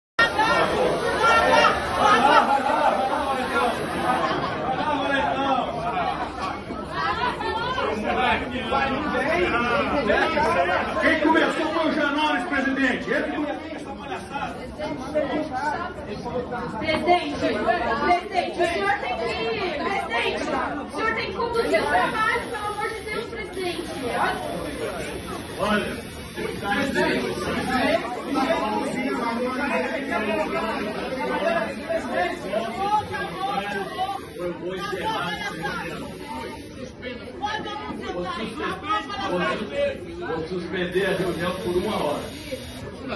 Deputado Janones denuncia ameaça de morte por Fraga no Congresso e registra ocorrência na PF.
André Janones afirma ter sido ameaçado por Alberto Fraga durante sessão no Congresso
O deputado federal André Janones (Avante-MG) usou sua rede social nesta quarta-feira (29) para comunicar que vai à sede da Polícia Federal para realizar uma ocorrência por ameaça de morte contra Alberto Fraga (PL). Segundo Janones, o deputado Fraga, aparentemente, estava armado com um revólver na cintura quando partiu para cima do parlamentar.